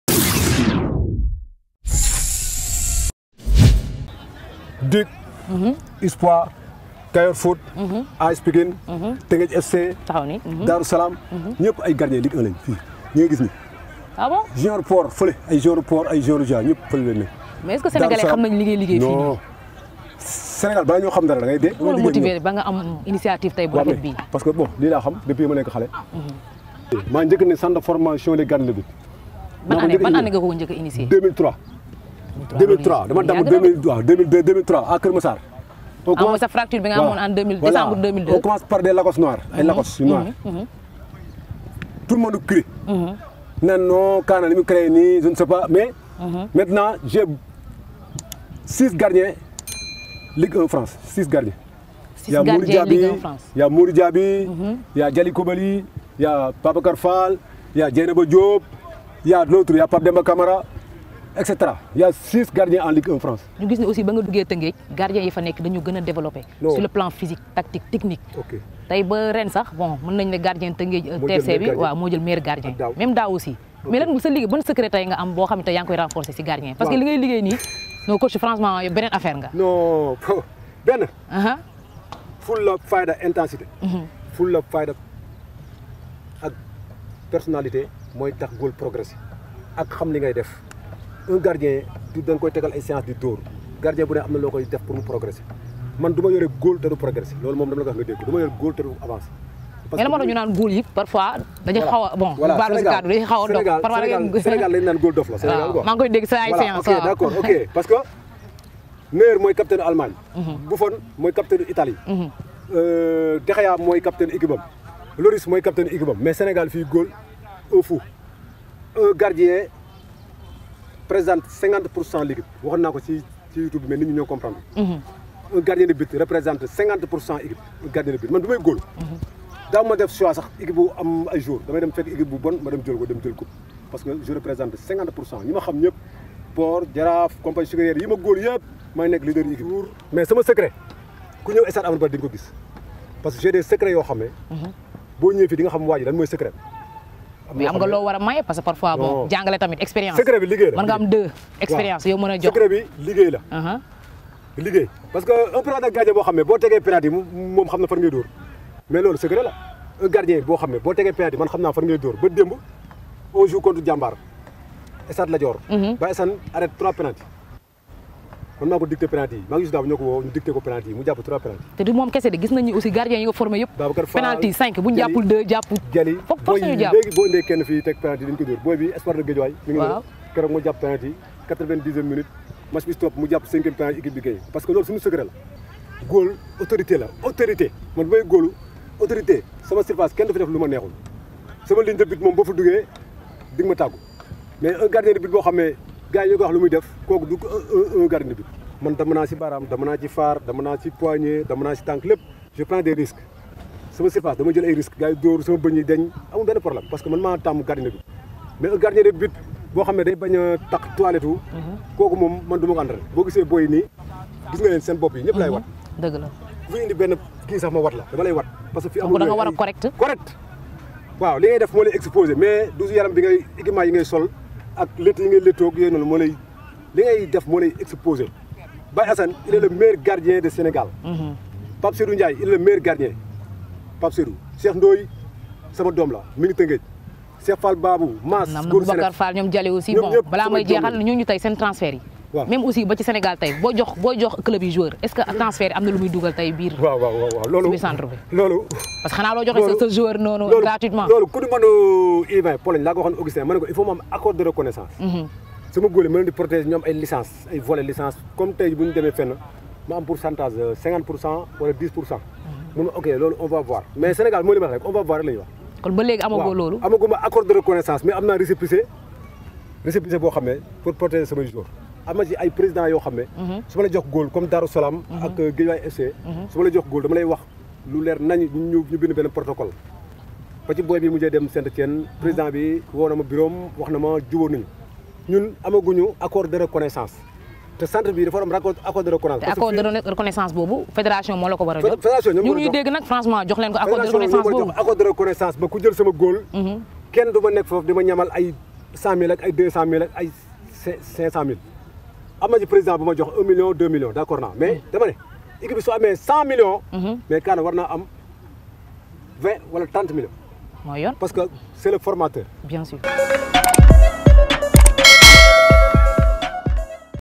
Kana Fall " J'ai 6 gardiens en L1 française, toujours pas d'accord de reconnaissance avec la FSF"
Entraîneur de gardiens de but , ayant ouvert le 1er centre de formation de gardiens au Sénégal, Kana peut être fier de son travail. Kobaly ndiaye, boubacar Fall,  Pape Demba Camara sont tous passés entre ses mains sans compter tous les grands gardiens actuels au Sénégal qui viennent s'aguerrir dans son centre. Malgré son expertise et le travail accompli, Kana Fall n'arrive pas encore à tirer les dividendes de son travail. Et l'ancien gardien de demander à la FSF de l'aider à obtenir un accord de reconnaissance  pour recevoir des indemnités lorsque les jeunes passés par son centre sont transférés"